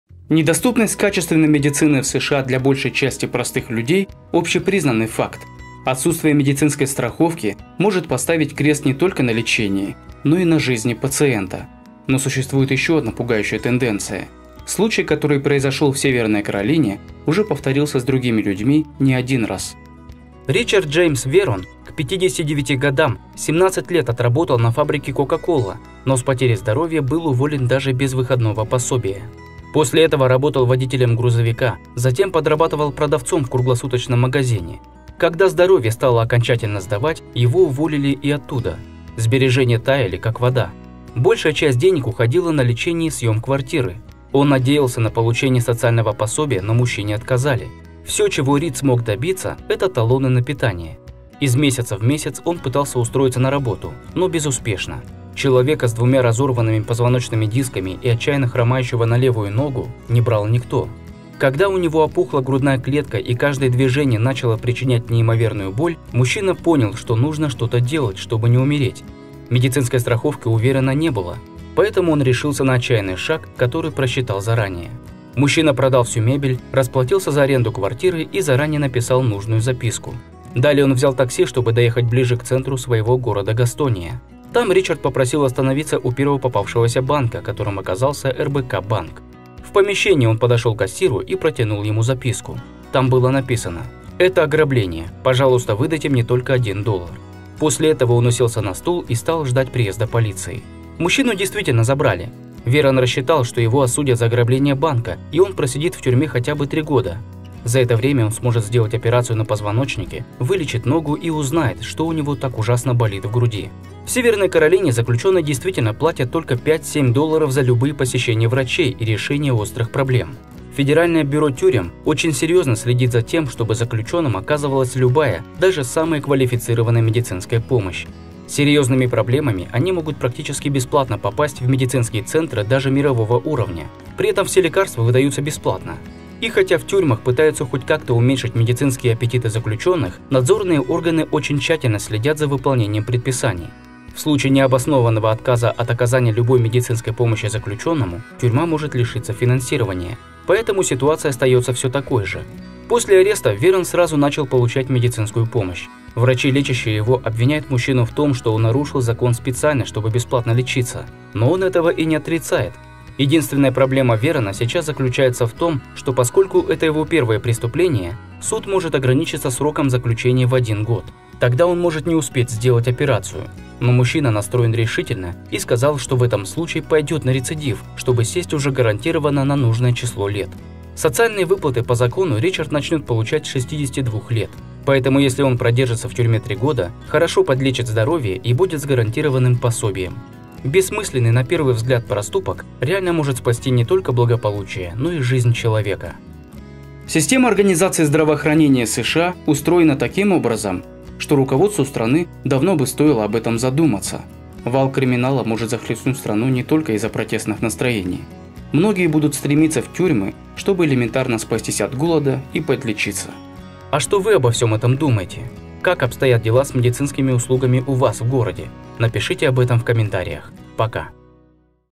Бездомный ограбил банк на 1 доллар, он хотел сесть в тюрьму и вот почему...
Ричард Джеймс Верон к 59-ти годам 17 лет отработал на фабрике Coca-Cola, но с потерей здоровья был уволен даже без выходного пособия. Сбережения таяли, как вода. Большая часть денег уходила на лечение и съём квартиры. Он надеялся на получение социального пособия, но мужчине отказали. Всё, чего Ричард смог добиться, это талоны на питание. Из месяца в месяц он пытался устроиться на работу, но безуспешно. Человека с двумя разорванными позвоночными дисками и отчаянно хромающего на левую ногу не брал никто. Когда у него опухла грудная клетка и каждое движение начало причинять неимоверную боль, мужчина понял, что нужно что-то делать, чтобы не умереть. Медицинской страховки у Верона не было, поэтому он решился на отчаянный шаг, который просчитал заранее...